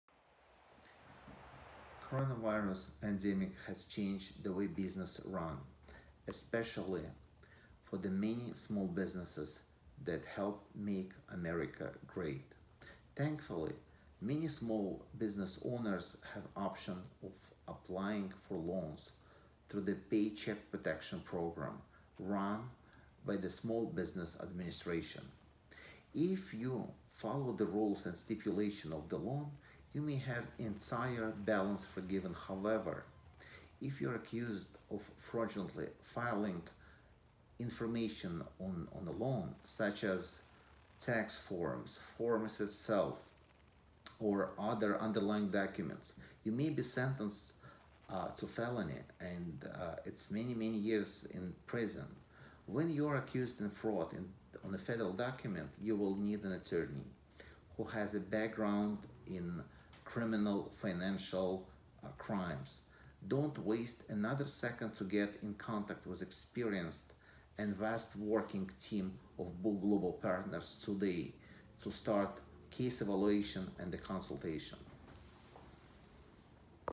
Post-COVID 19 PPP Lawyer in New York
Coronavirus has caused immense damage to New York, New Jersey, and the entire nation. Learn about how the government can prosecute small business owners for wrongdoings in PPP.